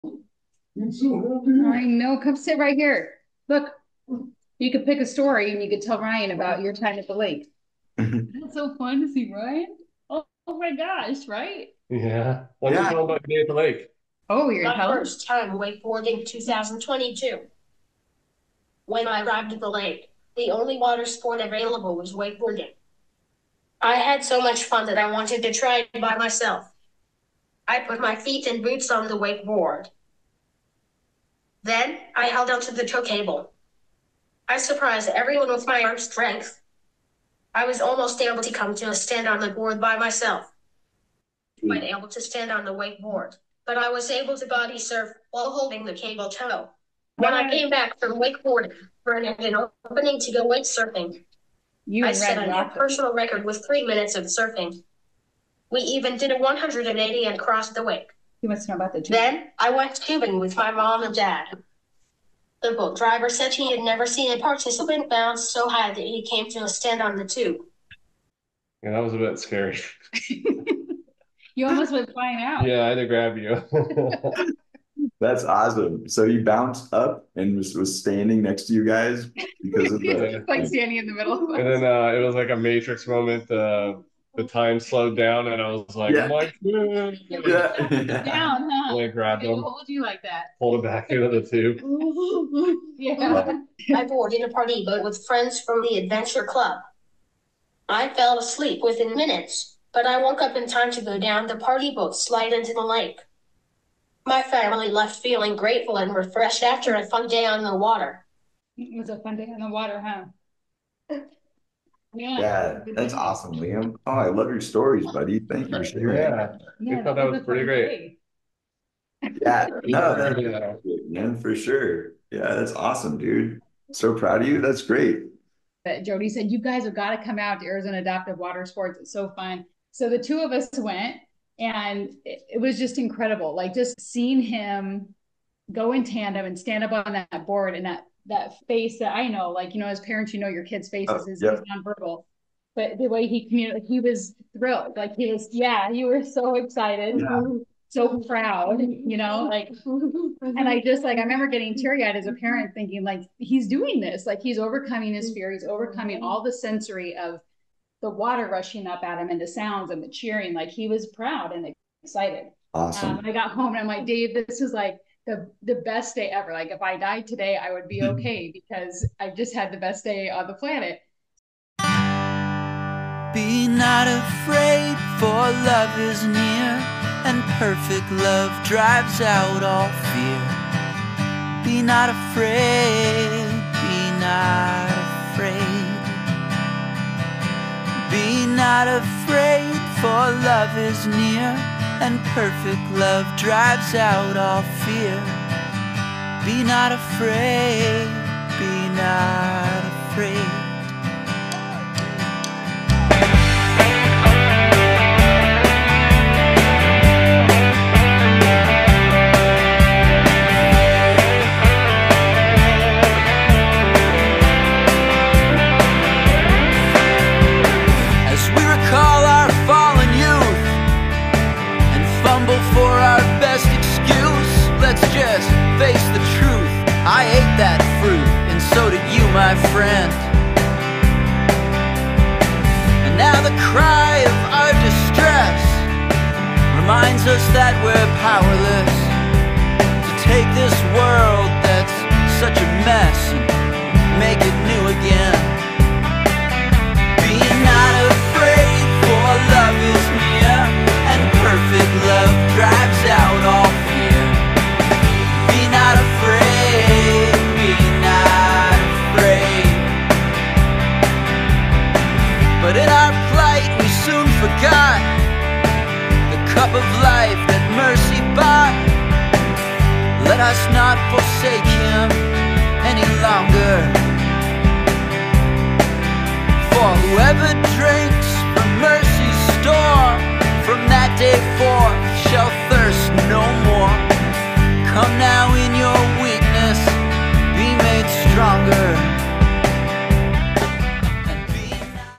Liam & Stacie Wylie - Be AAWSome AZ Adaptive Water Sports Athlete Story
In this Video Liam shares his favorite stories from the lake and Stacie recounts what it was like for her coming to the lake for the first time.